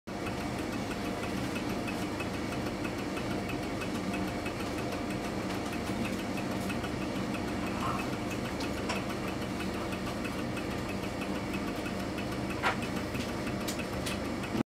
Winnie and the Stirling Engine
Winnie the Pooh tries to figure out how a Stirling Engine works.
I think he still hasn't grasped the importance of the 90-degree phase shift
between the power piston and the displacer piston.